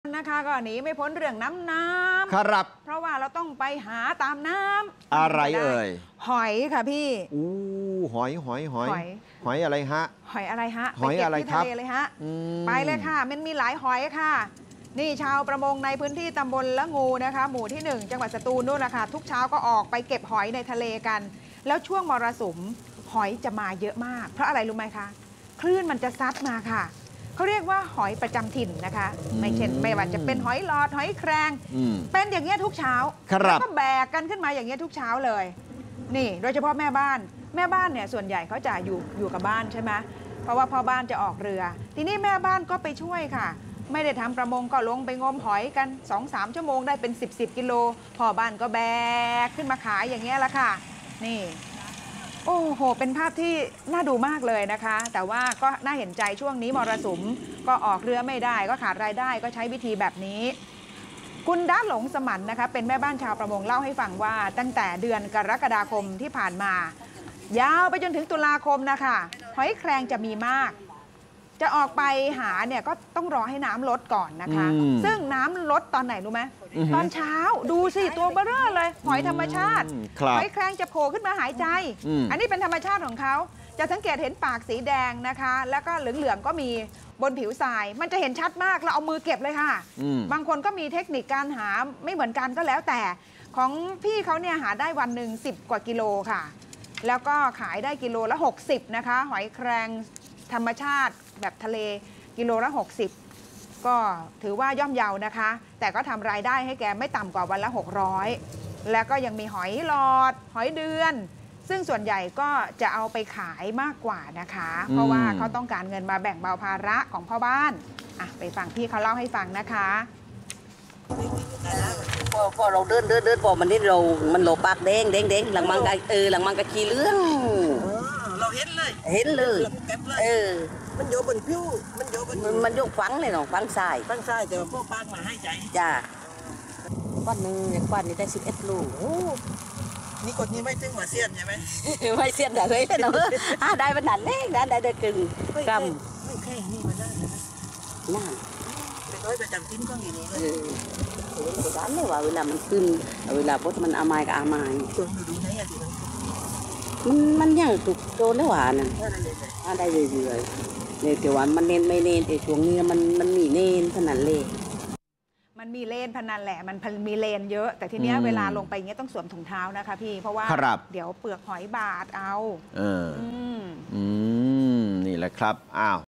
วิถีประมง หาหอยสร้างรายได้ช่วงมรสุม
ช่วงมรสุมก็ยิ้มได้ ชาวประมงพื้นบ้านสตูลออกหาหอยนานาชนิด โดยเฉพาะหอยแครงจะมีมาก เก็บขายสร้างรายได้ บางคนได้มากถึง 10 กก./วัน